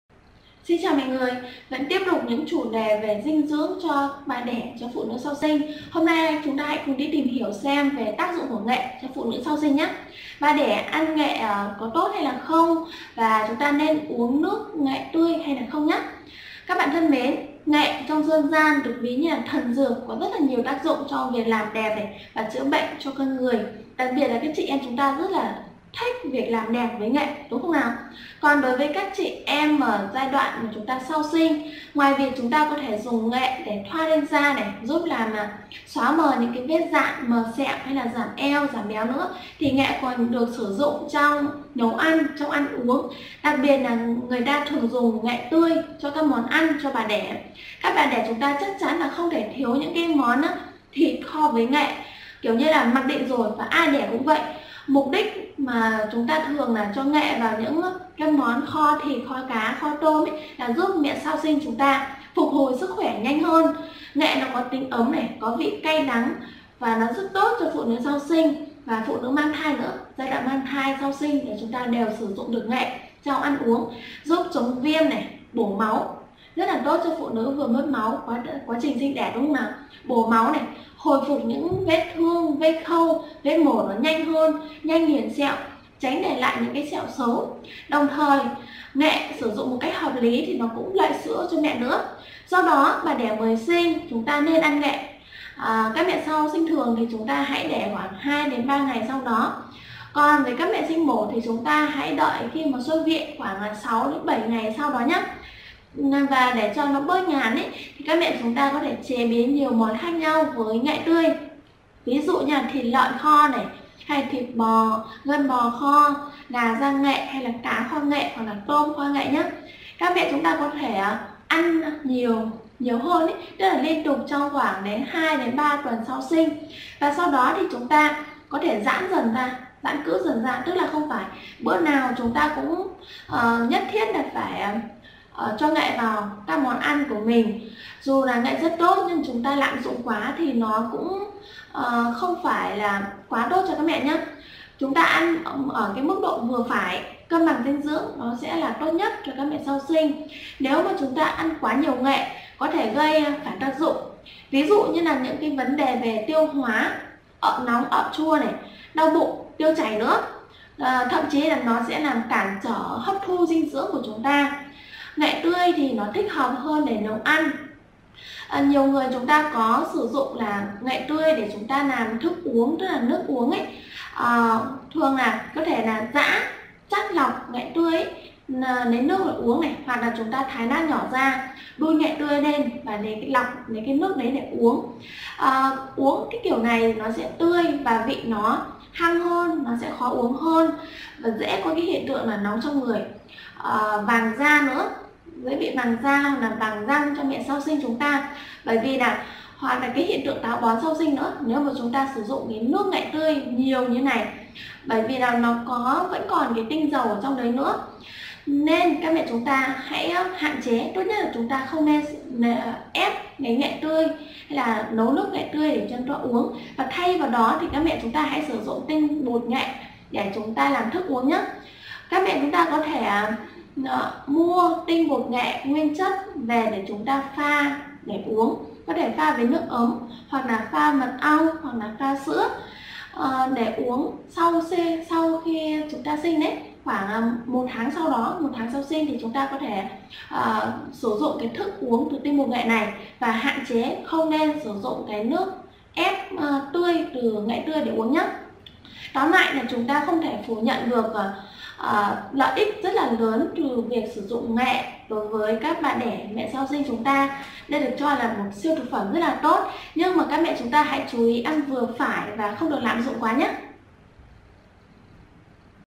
Bà đẻ phụ nữ mới sinh có nên ăn nghệ? Mẹ sau sinh uống nước nghệ tươi có tốt không?
Tác dụng của nghệ trong việc phục hồi sức khỏe sau sinh là điều không thể chối cãi. Không chỉ nấu thức ăn với nghệ tươi, nhiều bà đẻ mẹ bầu sau sinh còn uống tinh bột nghệ mật ong vừa giúp làm lành vết thương, đẹp da, chống viêm, tốt cho tiêu hóa.